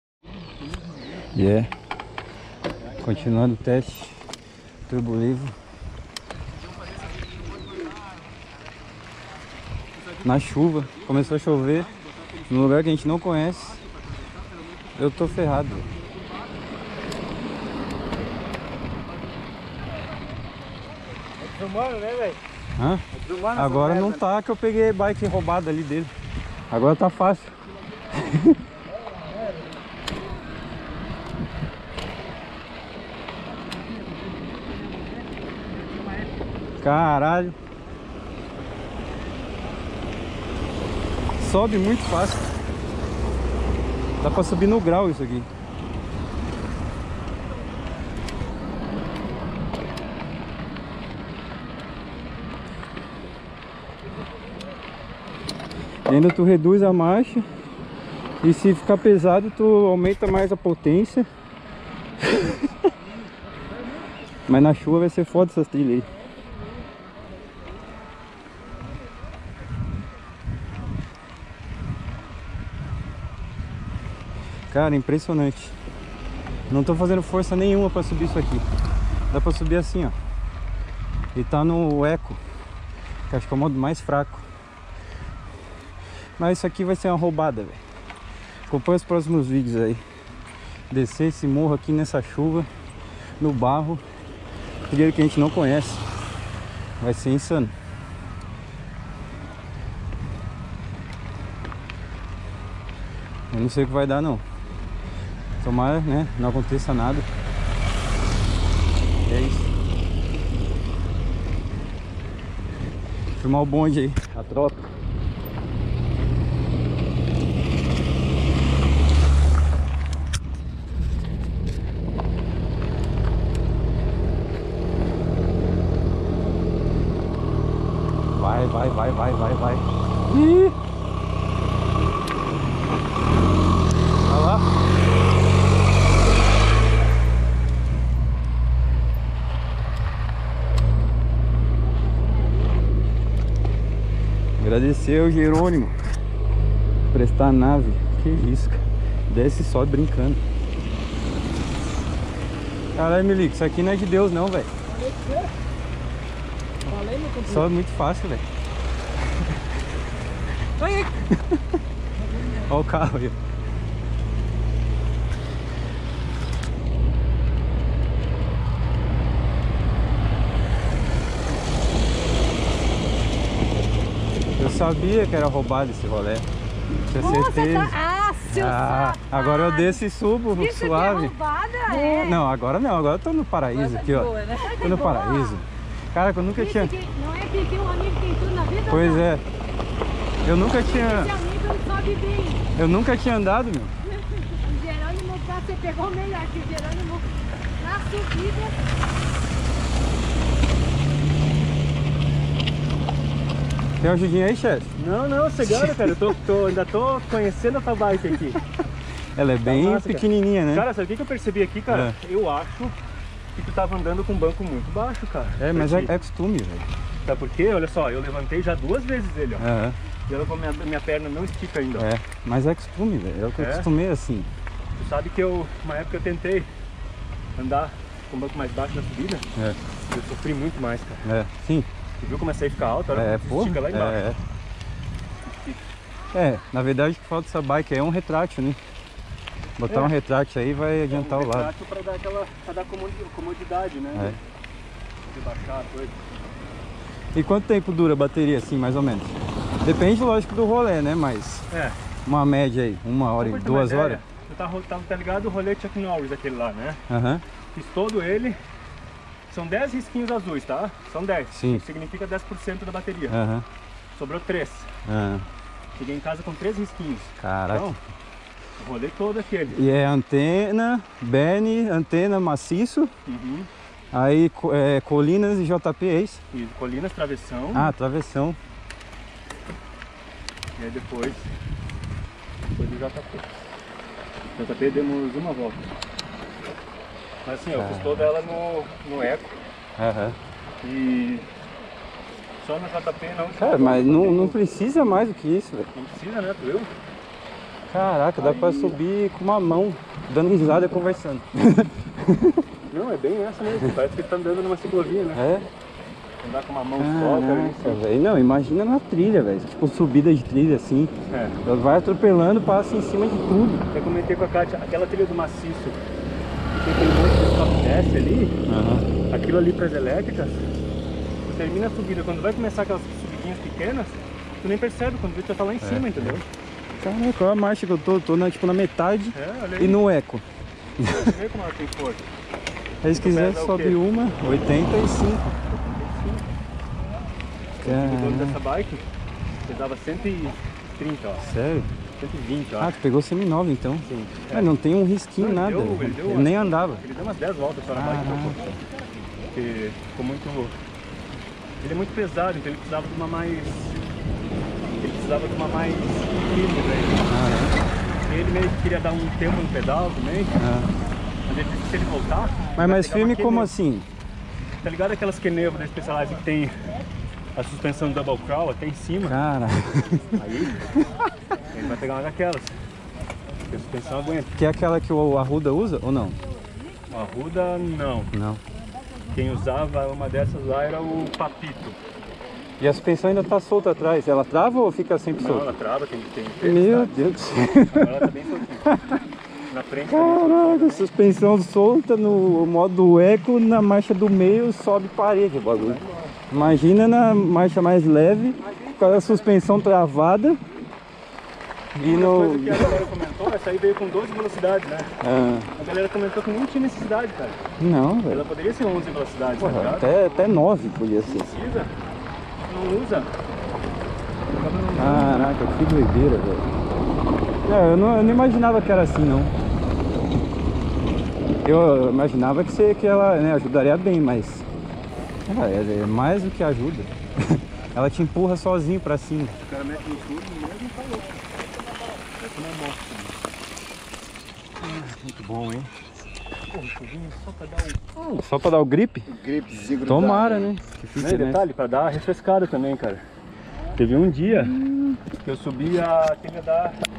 BIKE ELÉTRICA DE 60 MIL REAIS! SPECIALIZED TURBO LEVO! CONVERSANDO COM O DONO DO FOGUETE!
SALVIS BANDO DE LOUCO! SEGUE LÁ NO INSTAGRAM @OSAMAMOTOSOFICIAL CERTO? video de hoje parte dois do rolé na bike elétrica de 60 mil dinheiros, subindo um morro (a gopro engana ) na maior tranquilidade, sem esforço algum e no modo mais economico de força da bateria da bike! vou fazer um video falando melhor sobre as e bikes e trocando aquela ideia, tem mais um video no role com essa bike que semana que vem posto aqui! bom fim de semana! é nois!